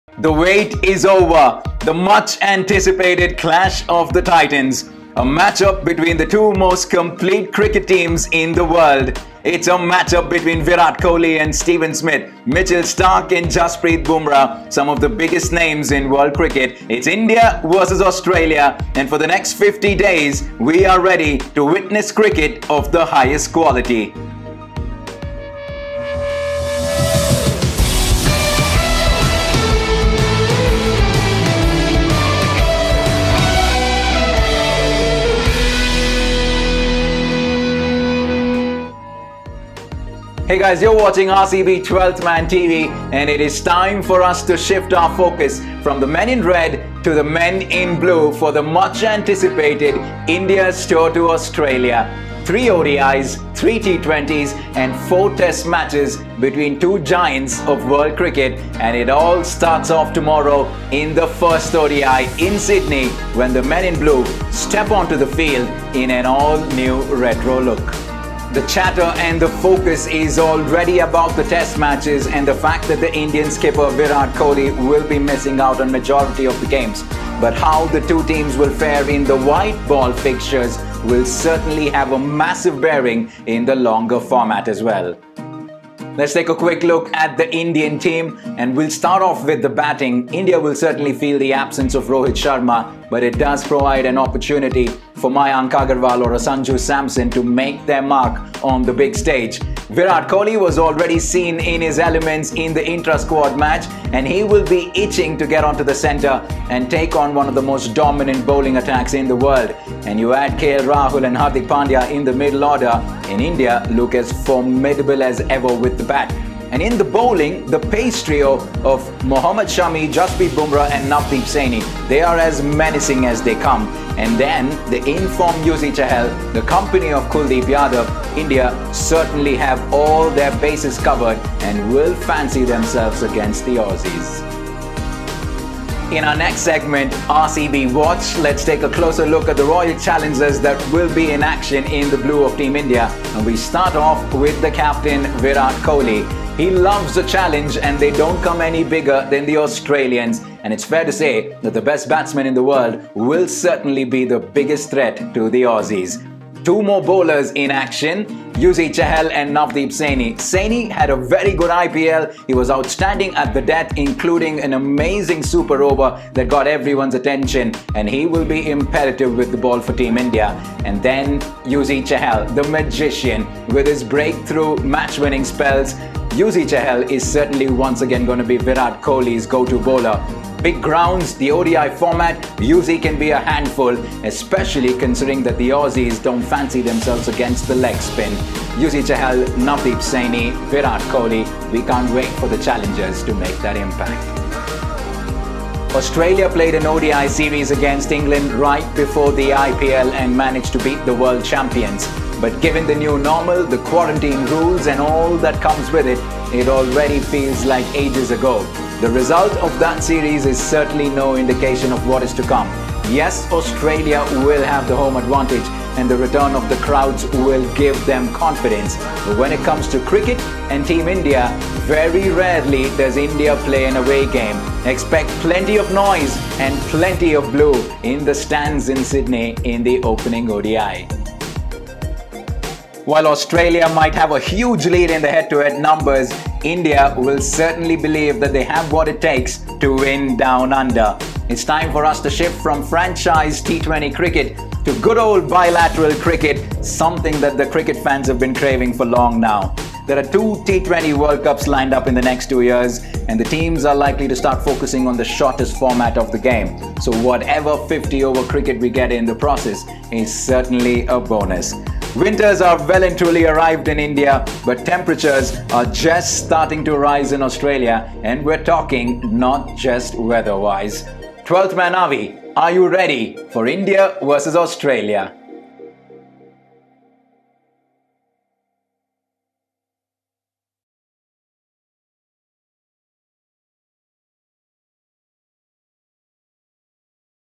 12th Man TV: Australia v India 1st ODI Preview
It’s the series we’ve all been waiting for. The clash of the titans! Here’s everything you need to know about the 1st ODI between Australia and India.